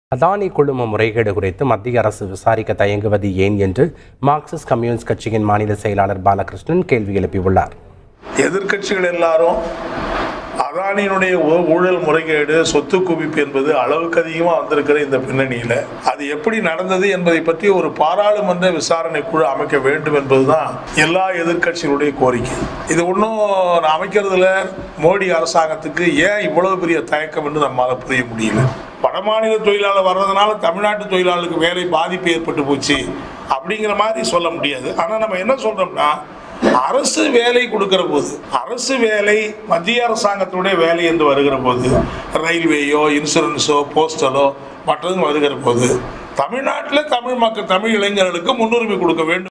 அதானி ஊழல்; மோடி அரசு விசாரிக்கத் தயங்குவது ஏன்?
அதானி ஊழல்; மோடி அரசு விசாரிக்கத் தயங்குவது ஏன்? - தோழர் கே.பாலகிருஷ்ணன், மாநிலச் செயலாளர் CPIM AdaniScam ModaniModelமார்க்சிஸ்ட் கம்யூனிஸ்ட் கட்சியின் செய்திகளைப் பெற தொடருங்கள்...